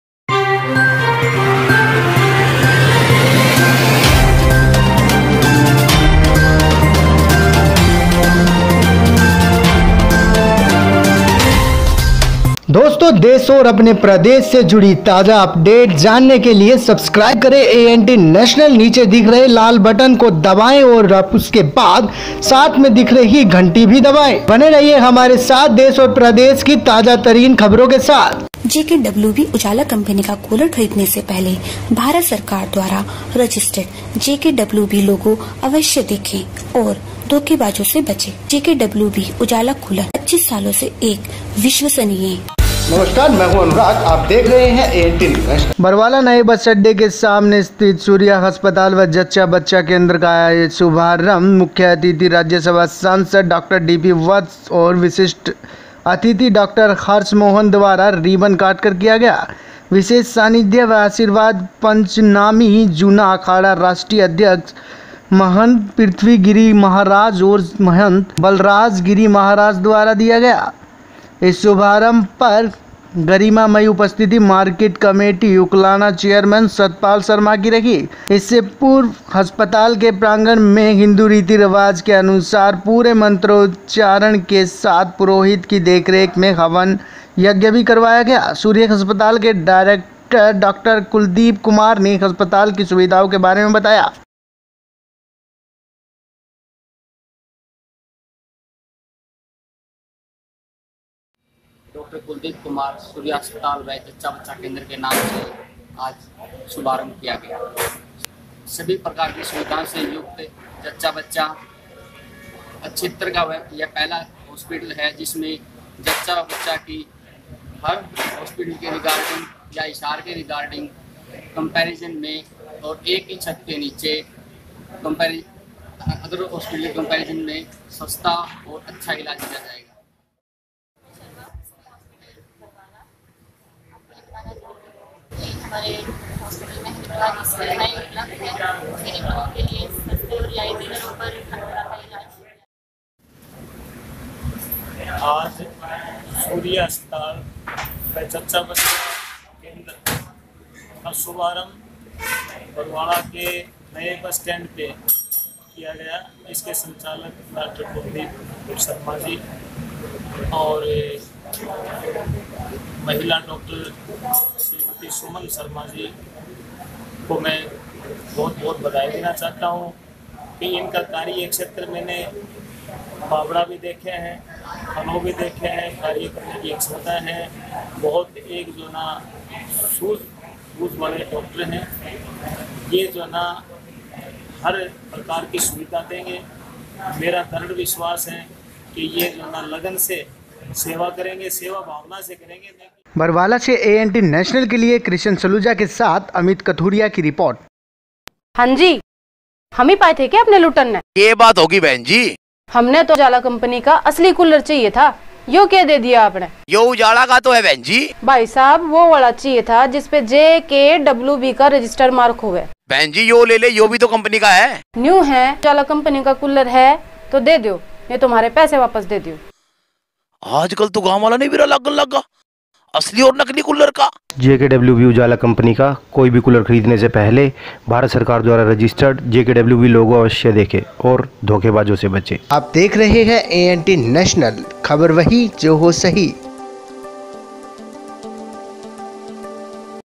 सांसद डॉ0 डीपी वत्स ने किया बरवाला के जच्चा बच्चा केंद्र का शुभारम्भ
"ANT national" ख़बर वही जो हो सही,सांसद डॉ0 डीपी वत्स ने किया बरवाला के जच्चा बच्चा केंद्र का शुभारम्भ